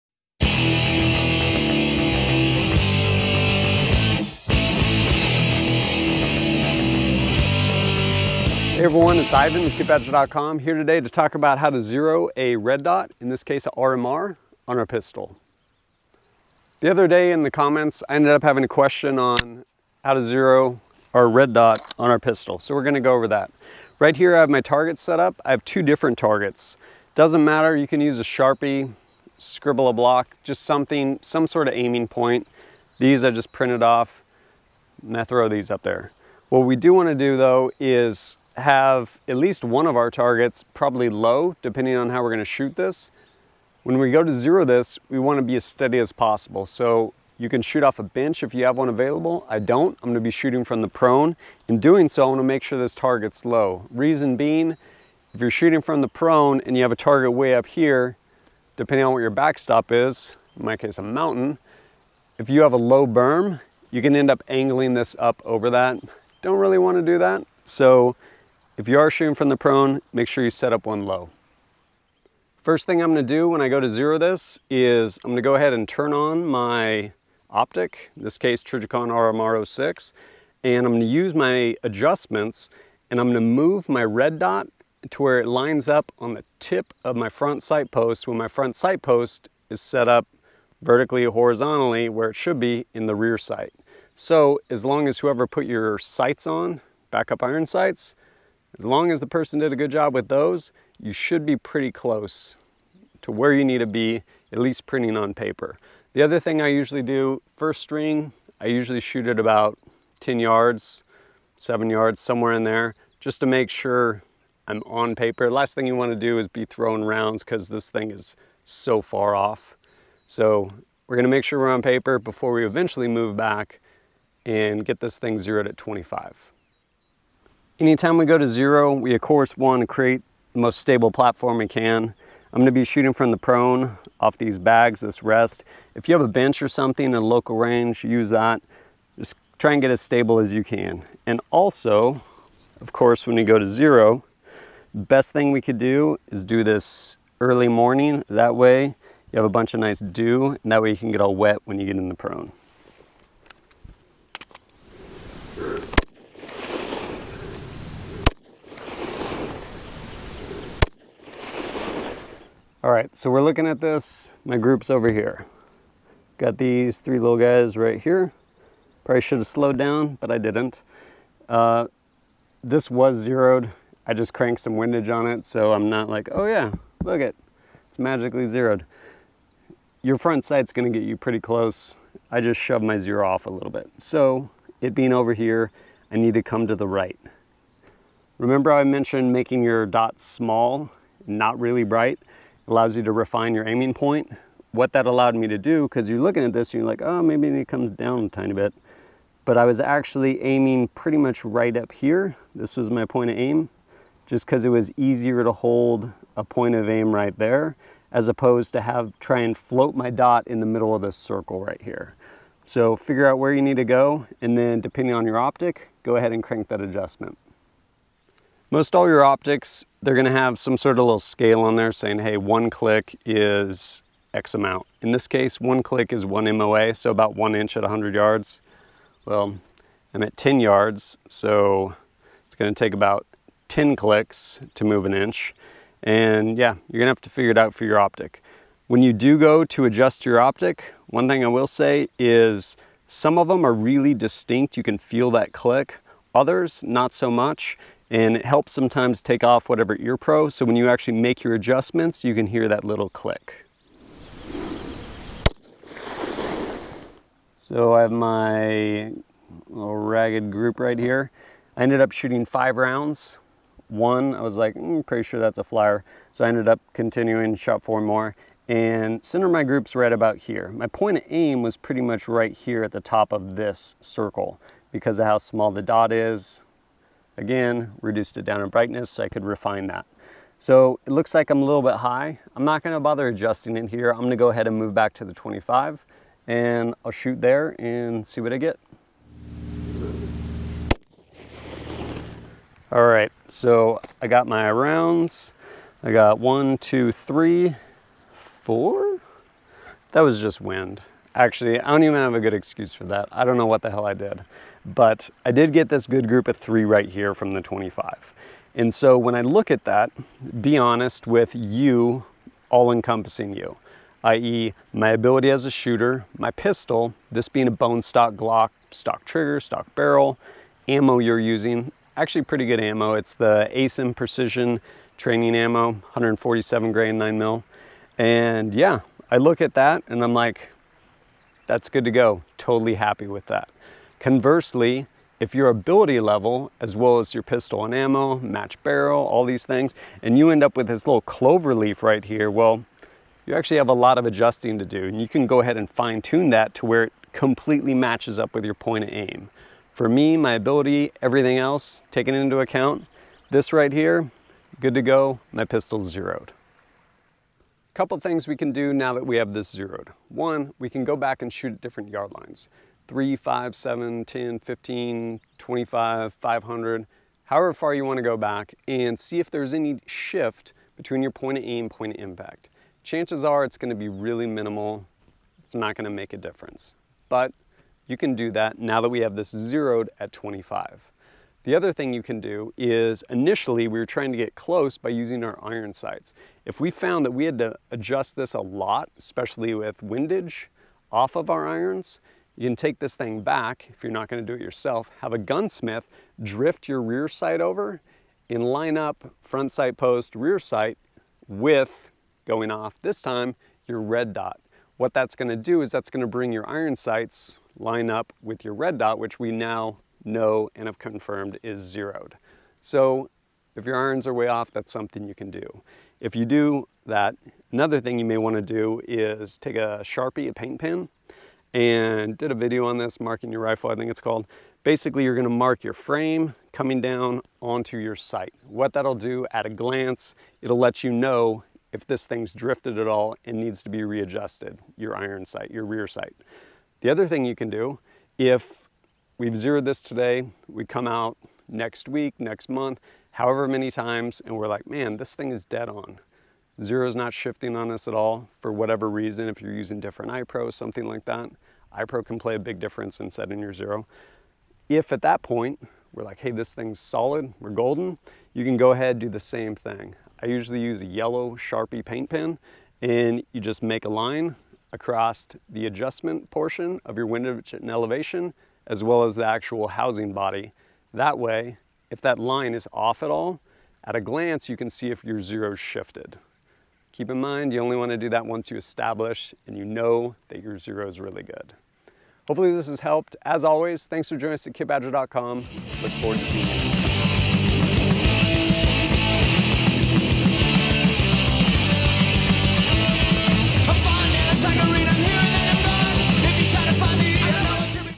Zeroing a Pistol Mounted Red-Dot Optic
In this video I go over how to Zero your Red Dot Optic on your pistol.  In my case, I am zeroing a Trijicon RMR 06 on my Glock 17.